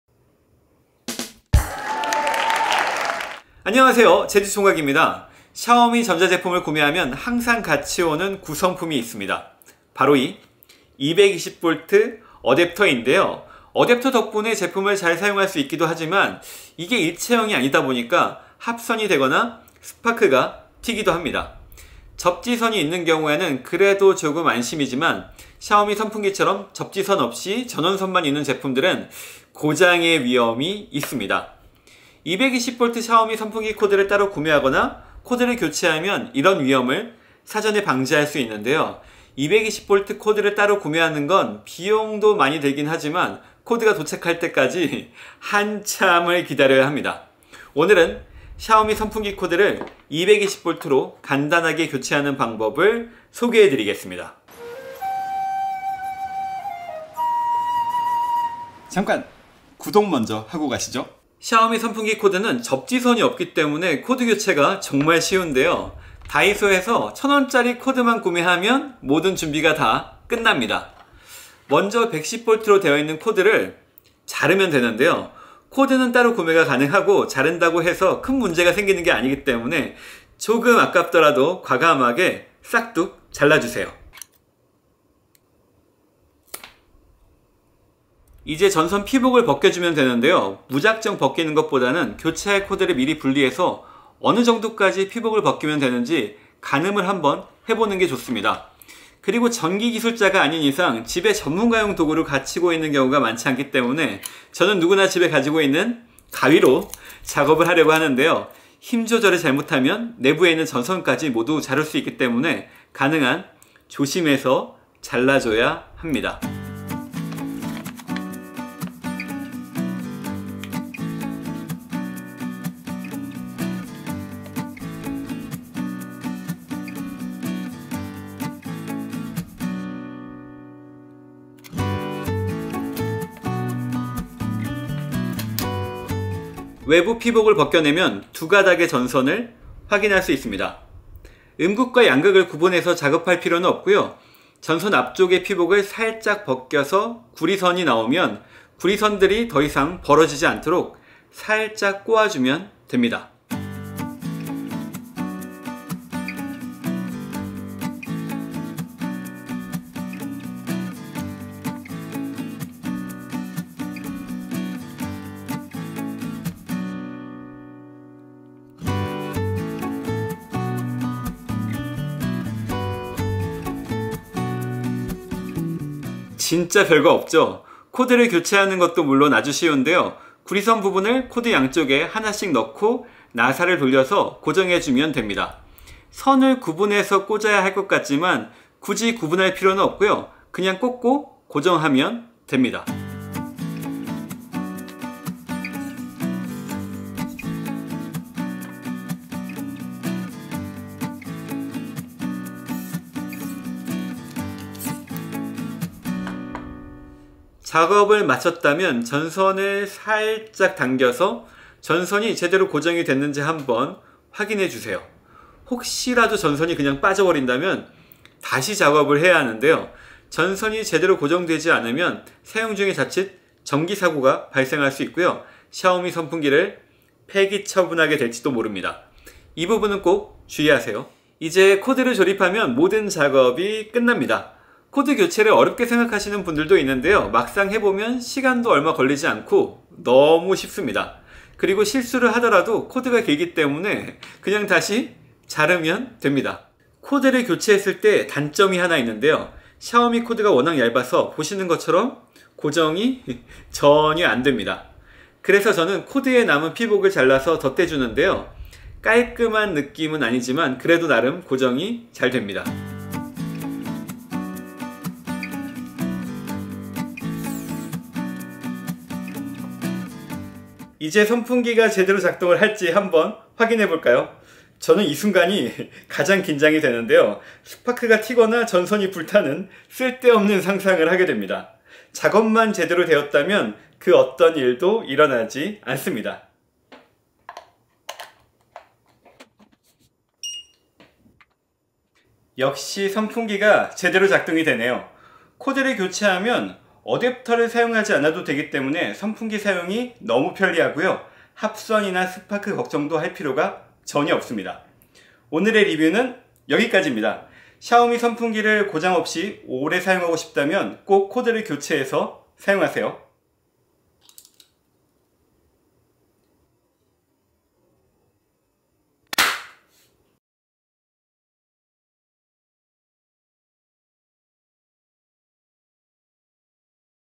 천 원으로 샤오미 선풍기 110볼트 코드 220볼트로 간단하고 쉽게 교체하기! How to replace xiaomi power cord
이번 영상은 샤오미 선풍기 사용을 돕기 위한 영상입니다
샤오미 선풍기 구매 리뷰를 보면
몇 일 사용하고 고장이 났다는 글들이 상당히 많습니다
좋다고 해서 구매했는데 너무 쉽게 고장이 나 버려서
황당하기보다는 화가 나는 상황이 되는거죠!

코드만 간단히 교체를 해도 이런 상황을 사전에 예방할 수 있는데요
이 영상만 따라오시면 샤오미 선풍기 코드교체
아주 쉽게 할 수 있습니다

접지전이 있는 제품 (코드 내부에 전선이 3개)은
다른 코드를 구매해서 교체하셔야 합니다
이 영상은 샤오미 선풍기처럼 접지선이 없는 제품을 위한 교체 영상입니다